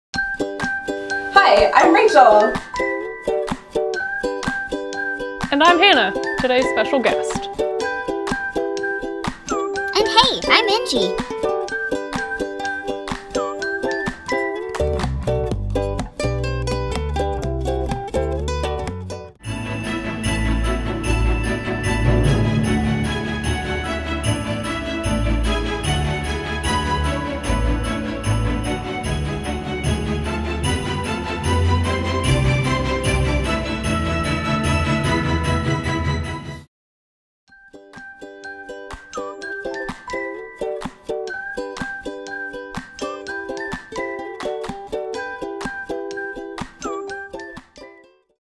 Episode 5 Sneak Peek - Gingerbread Geometry - Engie Does STEM
Hello Everyone! This will be our last Engie Does STEM episode of the year! Come back on December 18th to watch episode five! In Episode five you will learn about Geometry using Gingerbread Houses. Follow Rachael and special guest Hannah as they learn about Perimeter and Area.